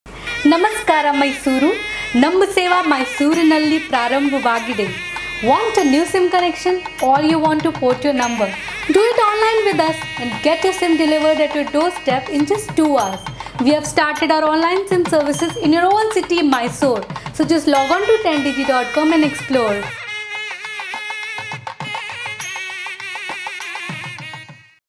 Big News For Mysore!
Big news!
We have started our services in Mysore.

No need to step out of your home, get your favourite SIM delivered at your doorstep.

Thank you all for your love
Team 10digi